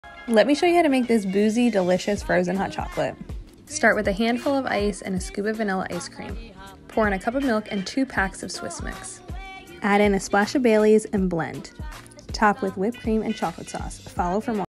Christmas Cocktail | Boozy Frozen Hot Chocolate Recipe
Boozy Frozen Hot Chocolate | Christmas Cocktail Idea

Ingredients:
- 2 packs of Swiss mix
- scoop of vanilla ice cream
- cup of milk
- dash of baileys
- ice
- whipped cream
- chocolate syrup

21+ Drink Responsibility